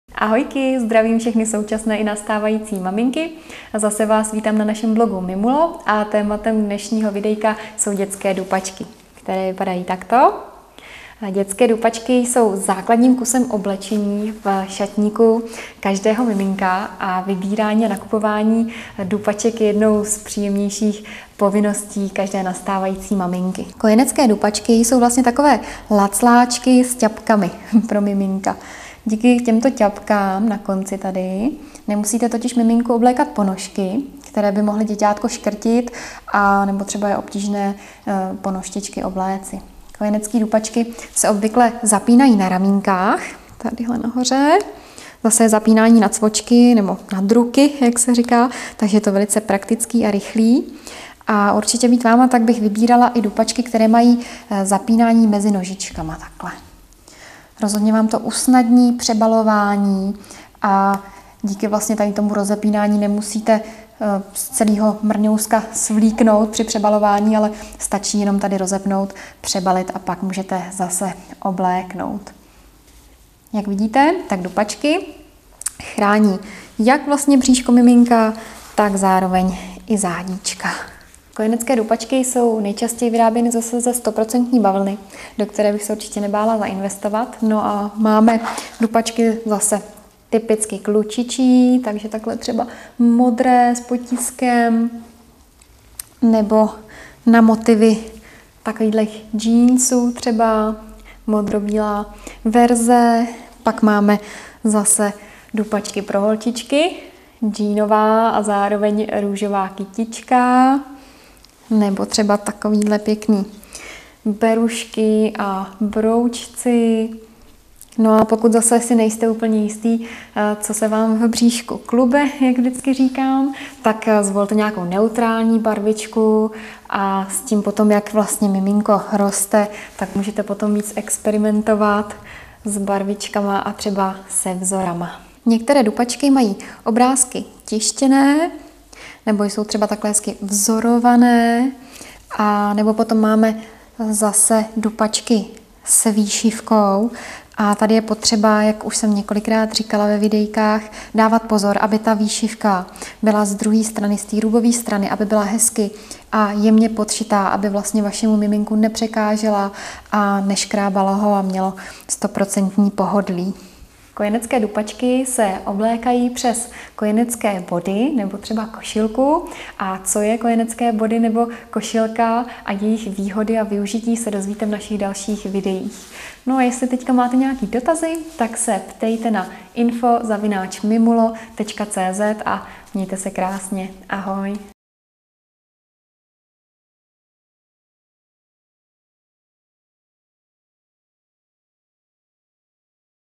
Kojenecké dupačky
Mnoho nastávajících maminek (prvorodiček) přesně neví, co takové miminko potřebuje. Jak tyto věcičky vypadají a k čemu jsou vlastně dobré. V tomto videu Vás seznámíme s kojeneckými dupačkami. Spolu s bodýčky patří kojenecké dupačky mezi nejoblíbenější a nejprodávanější oblečení pro miminka. Nezapomeňte je proto Vašemu malému miláčkovi taky pořídit. Uvidíte, že brzy oceníte jejich praktičnost. Miminko je může nosit jak přes den, tak i v noci na spaní. Každá máma jistě ocení zapínání na cvočky mezi nožičkami, což ji urychlí a velmi usnadní přebalování. Vybírat si můžeže z naší bohaté nabídky kojeneckých dupaček různých barev, provedení, velikostí. Velký výběr kojeneckých dupaček najdete na